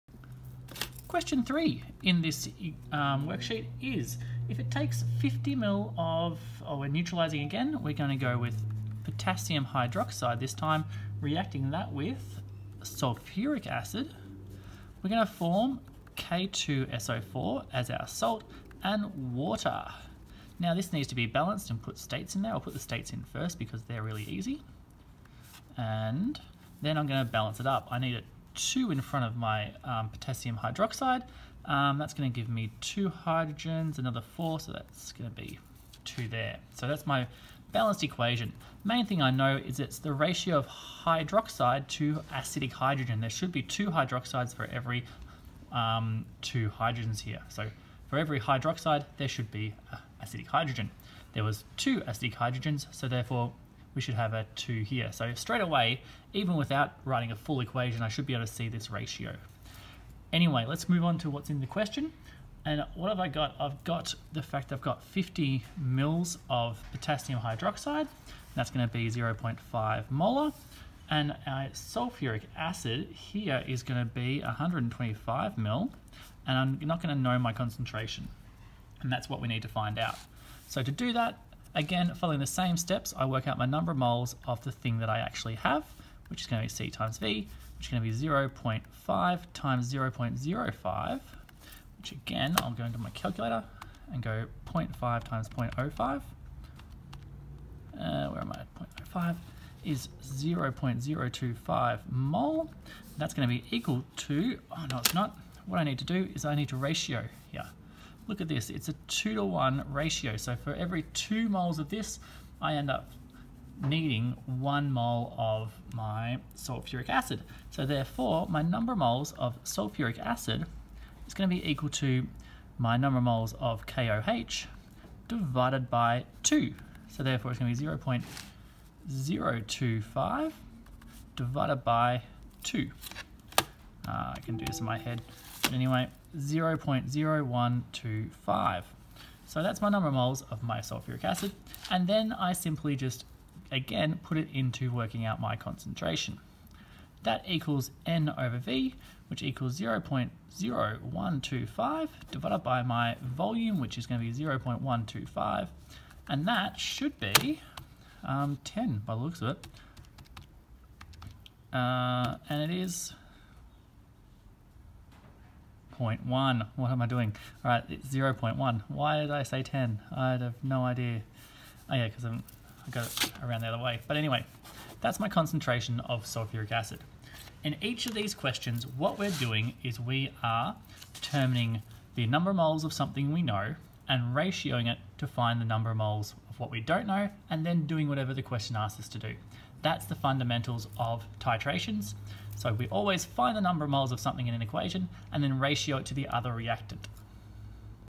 Simple Titration Style Question 3 (Acid / Base Calculation)
Question 3: If it takes 50 mL of 0.5 M KOH solution to completely neutralize 125 mL of sulfuric acid solution (H2SO4), what is the concentration of the H2SO4 solution?